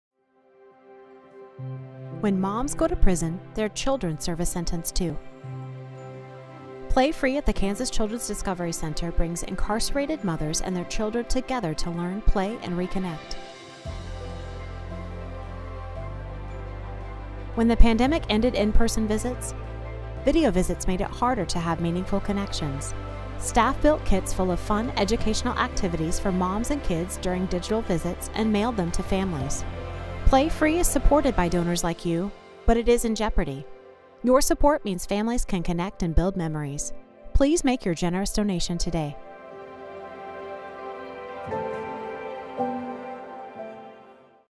Serving Children With Moms in Prison During a Pandemic
At the Kansas Children’s Discovery Center, we believe in the power of play. Because of the museum's special Play Free program, children with mothers in prison have opportunities to visit their moms in a safe, supportive environment. When the museum closed on March 13, 2020, the play stopped, but staff got to work bringing the museum experience into homes. Here’s the story of how, with support from donors like you, we connected mothers and children through play. Help is still needed.